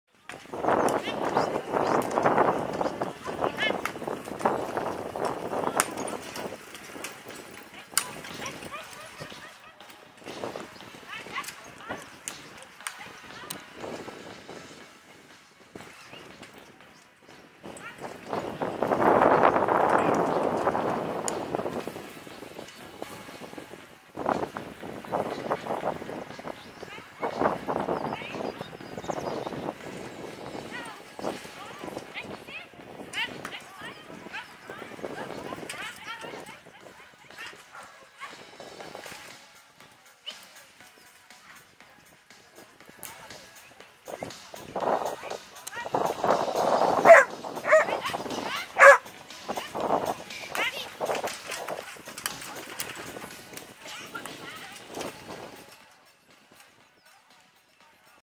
Fahrturnier Wettringen 24.4.2022
Ein kurzes Video einer Marathon-Fahr-Prüfung vom Fahrturnier in Wettringen
Sonntag